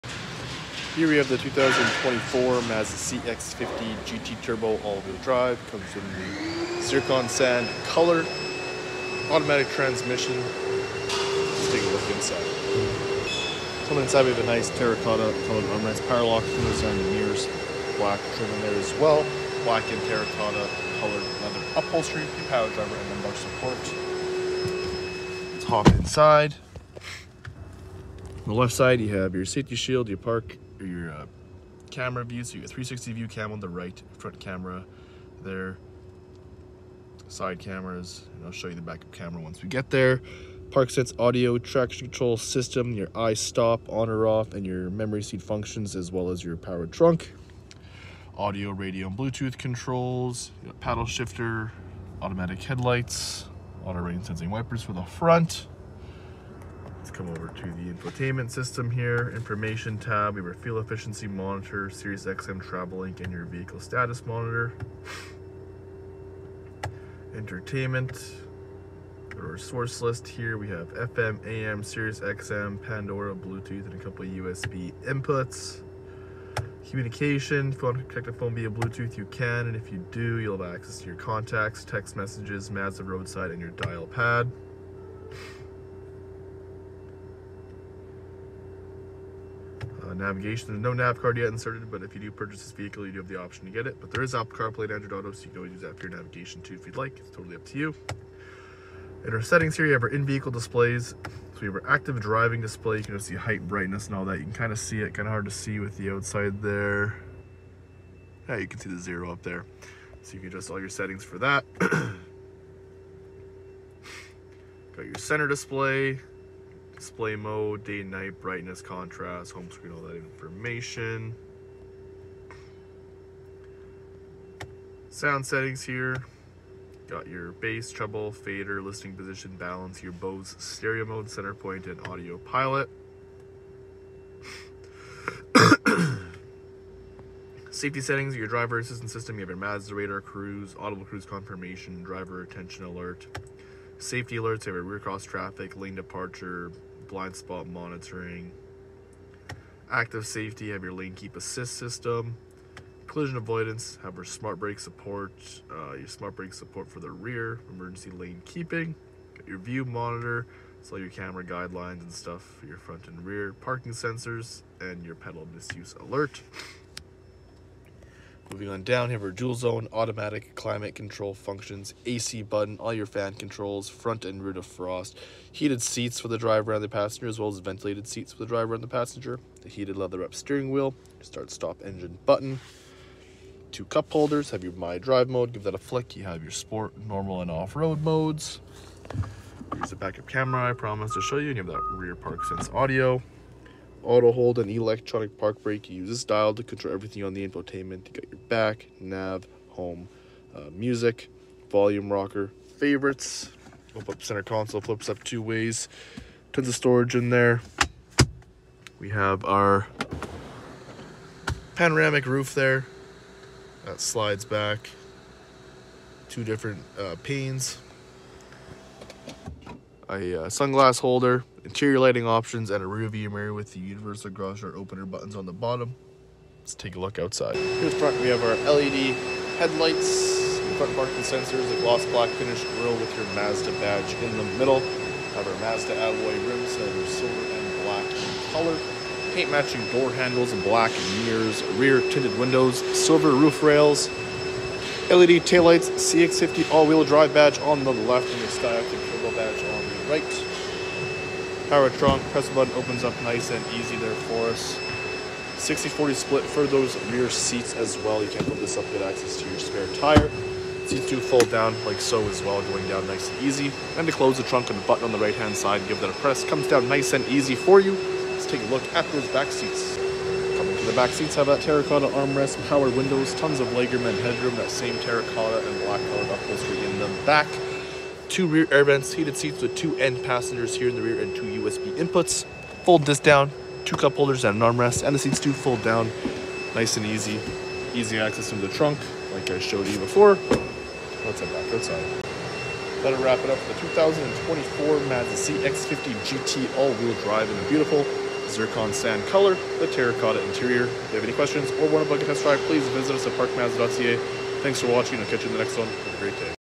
2024 Mazda CX-50 GT Turbo AWD (ZIRCON SAND METALLIC - TERRACOTA) Review - Park Mazda
Introducing the exhilarating 2024 Mazda CX-50 GT Turbo AWD, a harmonious fusion of innovation and style that redefines driving pleasure. Designed to captivate the senses and elevate your journey, this dynamic SUV seamlessly combines cutting-edge technology with Mazda's signature craftsmanship. With a spirited turbocharged Skyactiv-G 2.5L 4 Cylinder engine under the hood, the CX-50 GT Turbo AWD delivers a thrilling driving experience, blending power and efficiency effortlessly. Its advanced All-Wheel Drive system ensures confidence-inspiring traction on any road, empowering you to explore new horizons with poise.